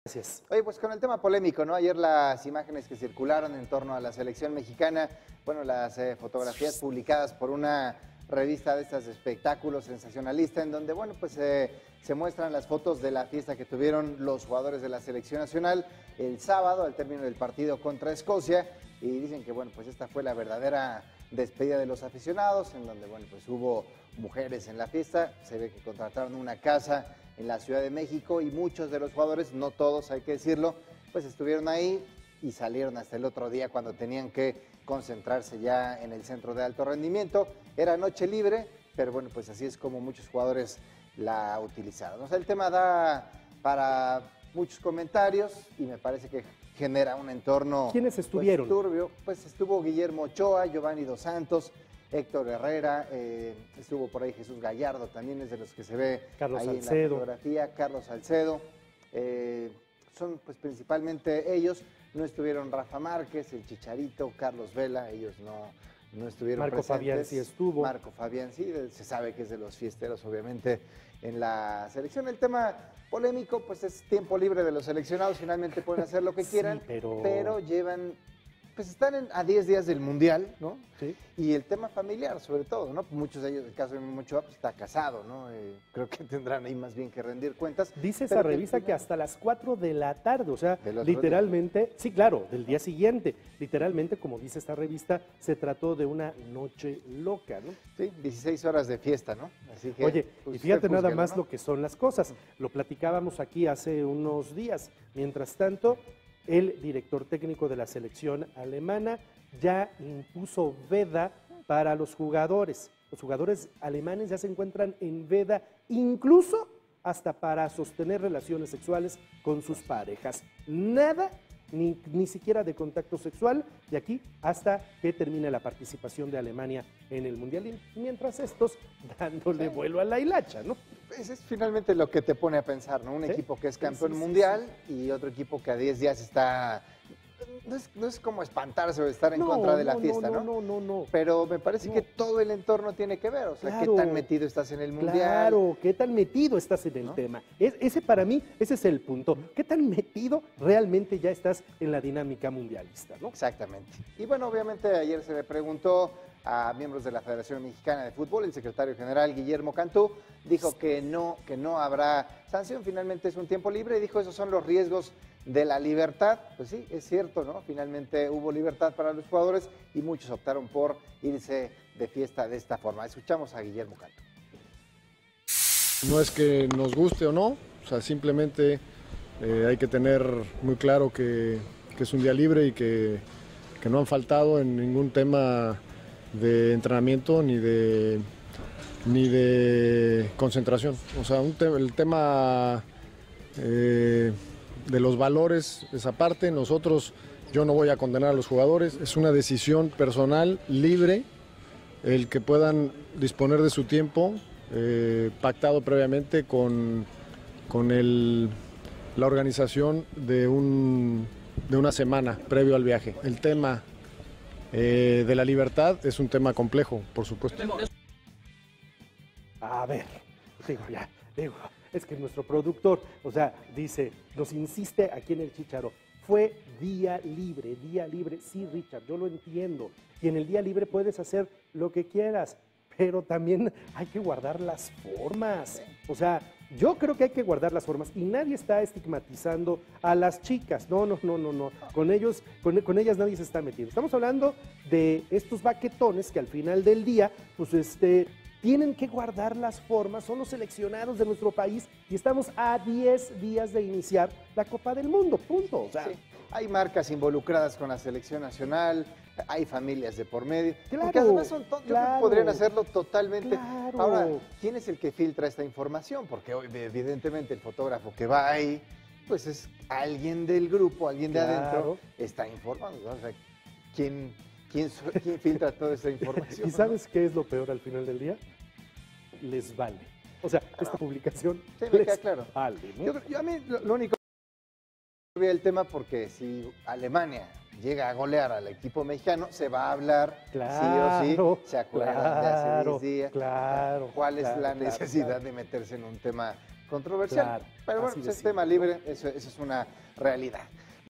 Nueva polémica de la Selección || Noticias con Juan Carlos Valerio
Nueva polémica de la Selección Mexicana de Fútbol por unas fotografías publicadas de una fiesta en su tiempo libre. Hubo mujeres involucradas y pone en predicamento a los futbolistas, sin embargo, no habrá sanciones.